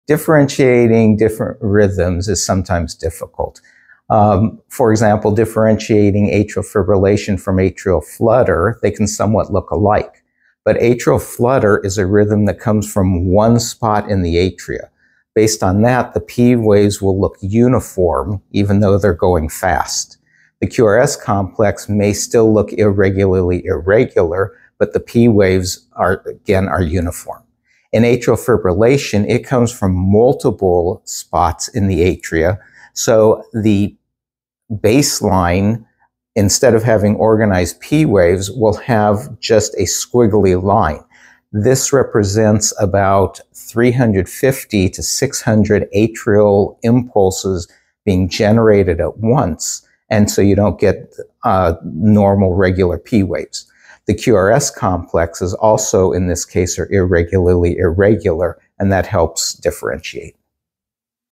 Afib vs Aflutter on ECG | How to Tell the Difference
Afib and Aflutter often look similar on an ECG — but they come from different places in the heart. 🫀
Dr. Scott Siegal explains how to distinguish:
➡ Aflutter = single atrial point
➡ Afib = multiple atrial points

🎓 Build confidence with interactive, CME-accredited ECG training at ECGedu.com